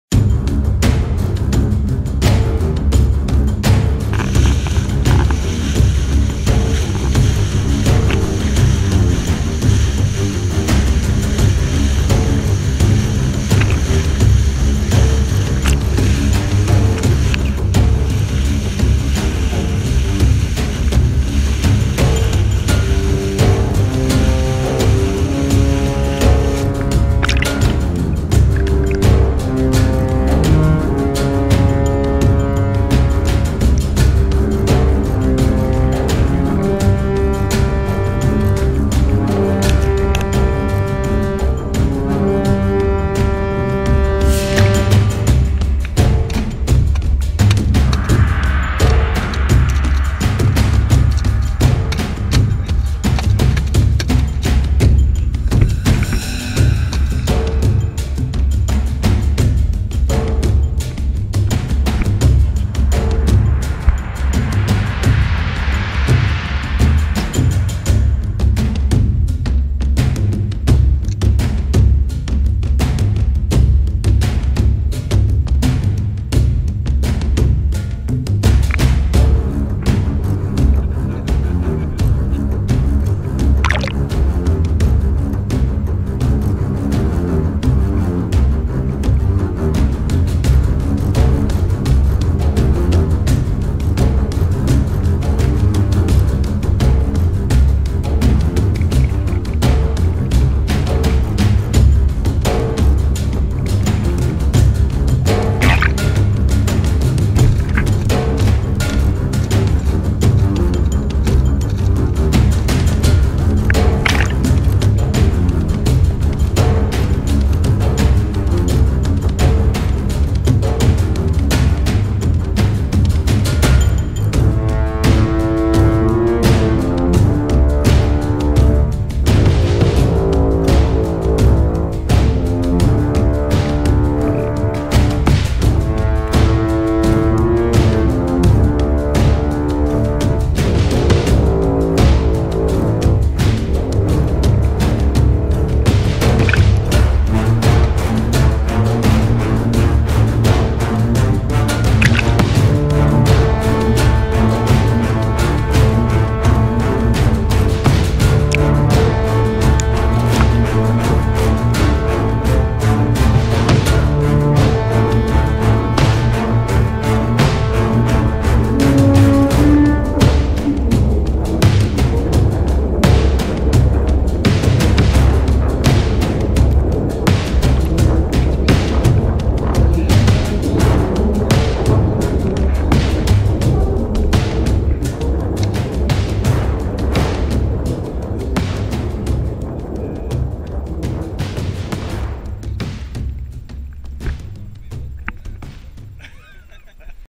Catching Atlantic halibut (Hippoglossus hippoglossus) and Atlantic cod (Gadus morhua)
Kveite / Helleflynder samt Torsk i Bodø, Norge.